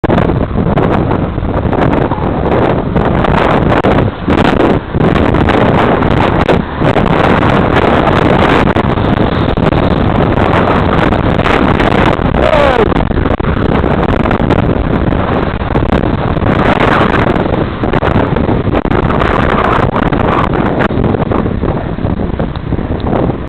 KB04KAE - Pointless, Dangerous Close Pass
This woman left a big gap when the traffic set off, so I was able to change lanes (checking back and indicating correctly). She didn't seem to like the fact that I had got in front of here, so did this very dangerous manoeuvre which allowed her to wait in the queue at the light for just a few seconds longer.

(I have a rear camera on order to provide more complete evidence in cases like this.)